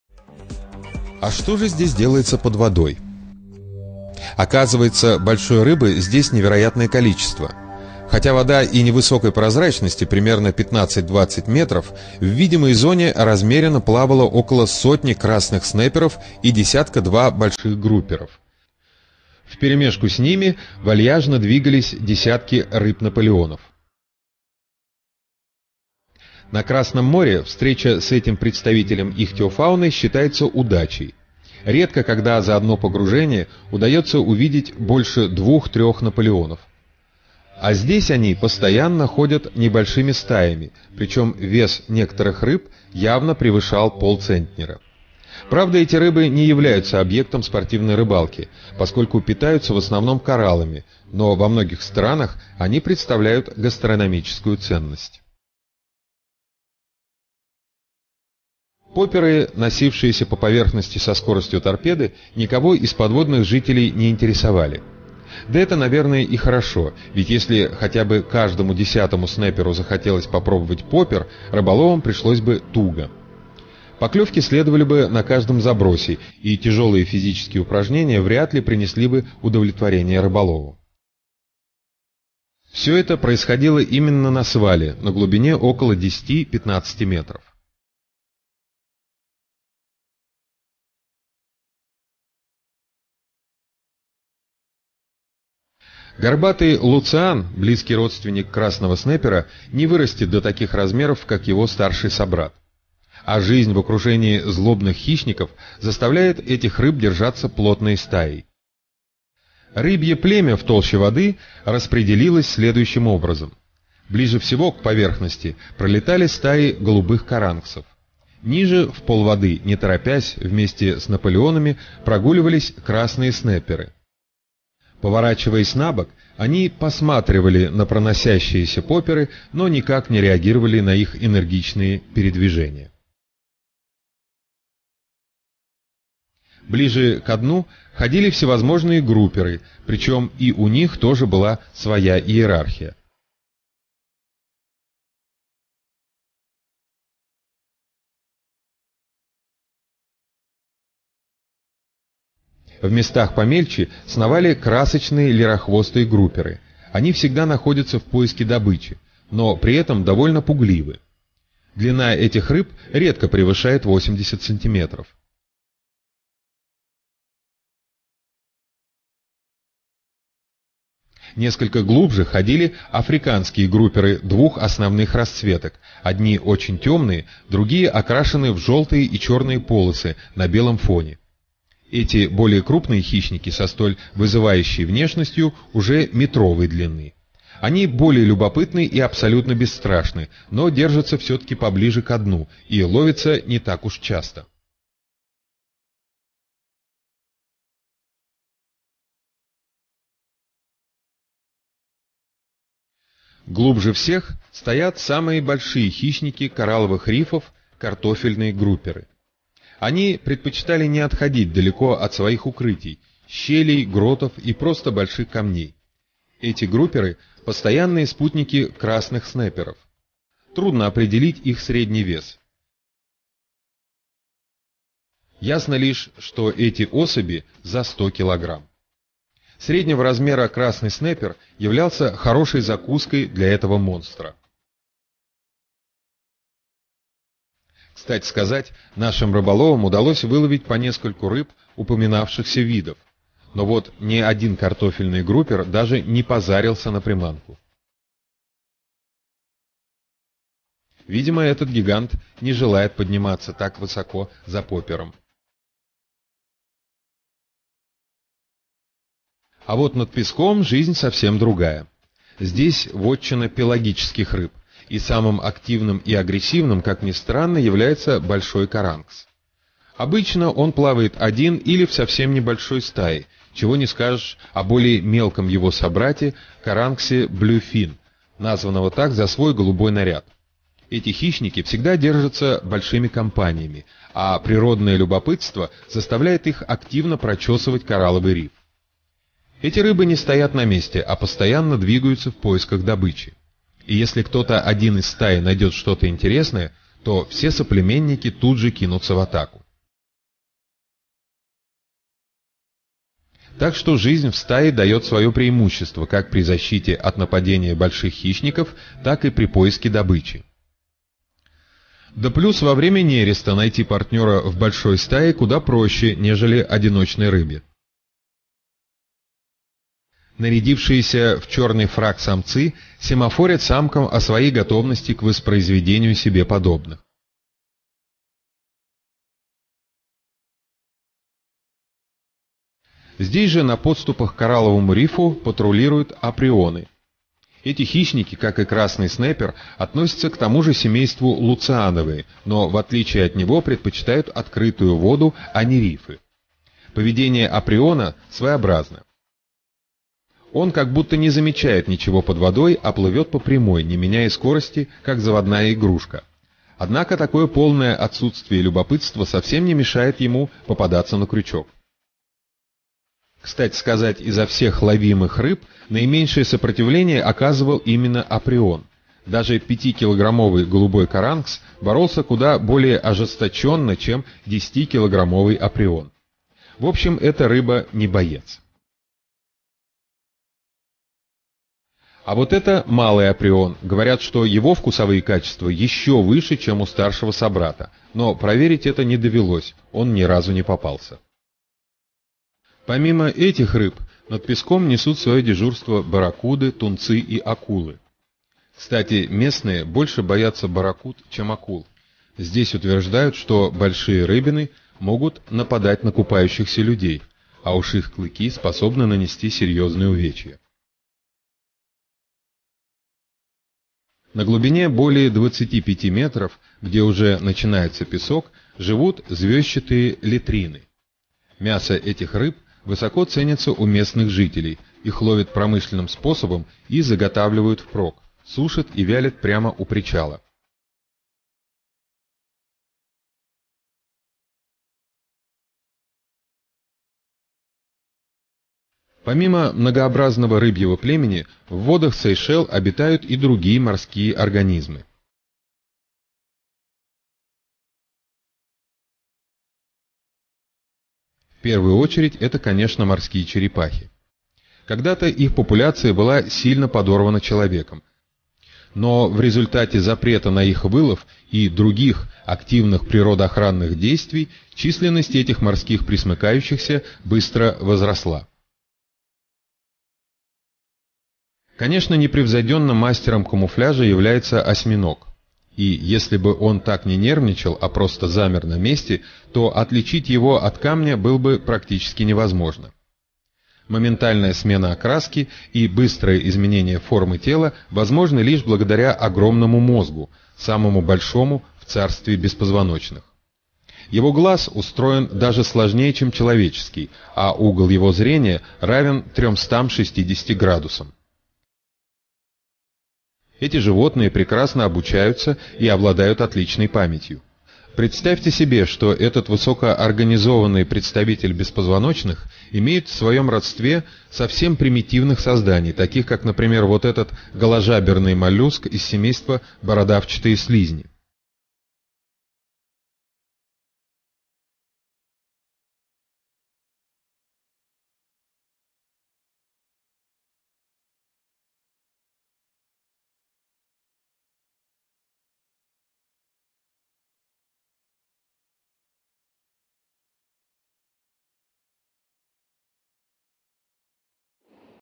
Подводные съемки на Сейшелах. Seychelles underwater
Съемки проводились во время рыболовного тура на Сейшельские острова. Их организовали редакция журнала "Рыболов-Elite" и туристическая компания Geo из Питера.